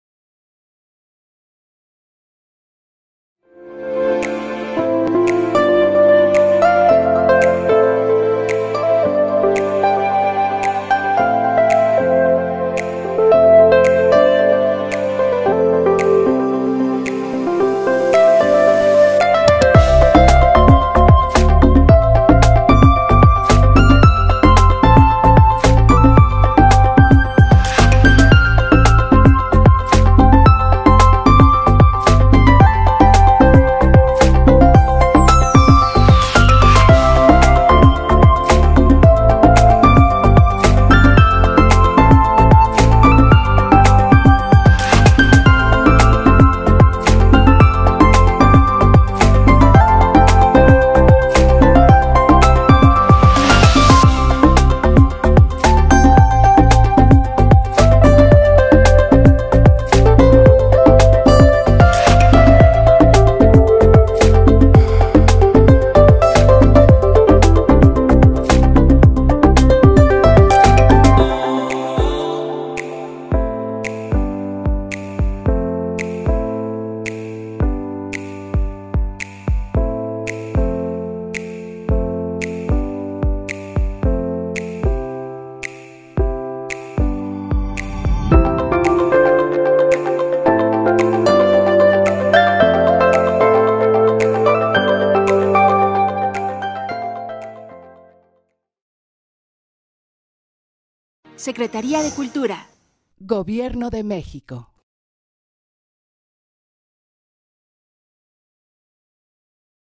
Decálogo de prevención para tu visita al Helénico
Nos estamos preparando para brindarte una gran experiencia, para eso necesitamos de tu ayuda para que tu estancia sea segura y amena.